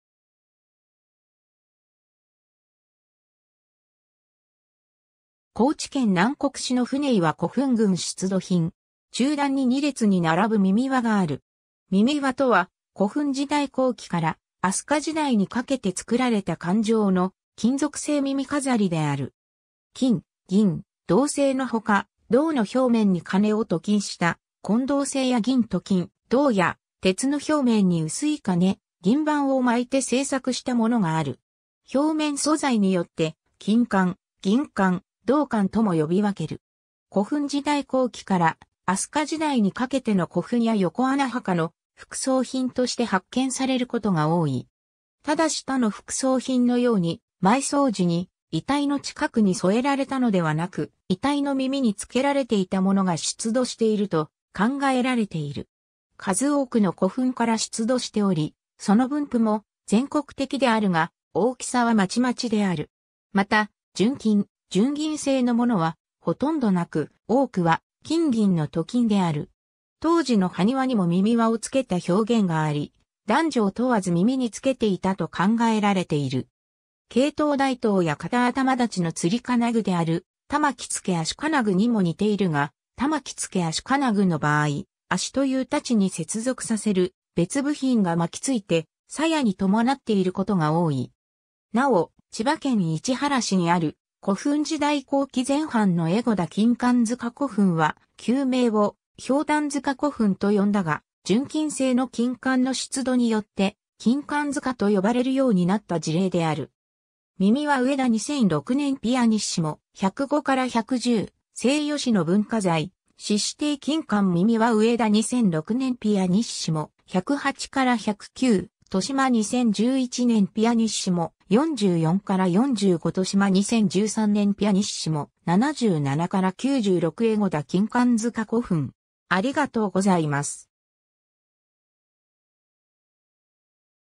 耳環
高知県南国市の舟岩古墳群出土品。
中段に2列に並ぶ耳環（金環・銀環）がある。
（高知県立歴史民俗資料館） 耳環（じかん）とは、古墳時代後期から飛鳥時代にかけて造られた環状の金属製耳飾り（イヤリング）である。
金・銀・銅製のほか、銅（または青銅）の表面に金を鍍金した金銅（こんどう）製や銀鍍金、銅や鉄の表面に薄い金・銀板を巻いて製作したものがある。
表面素材によって「金環」・「銀環」・「銅環」とも呼び分ける。
 古墳時代後期から飛鳥時代にかけての古墳や横穴墓の副葬品として発見されることが多い。
ただし他の副葬品のように、埋葬時に遺体の近くに添えられたのではなく、遺体の耳に着けられていたものが出土していると考えられている。
数多くの古墳から出土しており、その分布も全国的であるが、大きさはまちまちである。
また、純金・純銀製のものはほとんど無く、多くは金銀の鍍金である。
当時の埴輪にも耳環を付けた表現があり、男女を問わず耳につけていたと考えられている。
 圭頭大刀や方頭大刀の吊り金具である「環付足金具」にも似ているが、環付足金具の場合「脚」という大刀に接続させる別部品が巻き付いて、鞘に伴っていることが多い。
 なお、千葉県市原市にある古墳時代後期前半の江子田金環塚古墳は、旧名を「瓢箪塚古墳」と呼んだが、純金製の金環の出土によって「金環塚」と呼ばれるようになった事例である。
 ^ 耳環（金環・銀環・銅環）（三重県総合博物館） ^ 上田 2006年 pp.105-110 ^ 西予市の文化財・市指定 金環（愛媛県西予市サイト） ^ 耳環（神戸市埋蔵文化財センター） ^ 上田 2006年 pp.108-109 ^ 豊島 2011年 pp.44-45 ^ 豊島 2013年 pp.77-96 ^ 江子田金環塚古墳（市原市埋蔵文化財調査センター・遺跡ファイル）